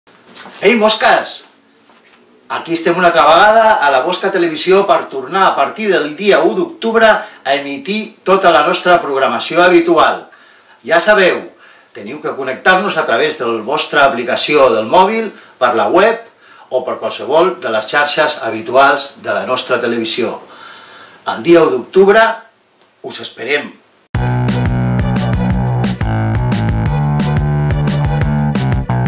A l'octubre torna la programació a LAMOSCATV
A l'octubre torna la programació a LAMOSCATV, tots els programes, debats, concerts i cinema. La podeu seguir per la nostra web o descarregant l'App.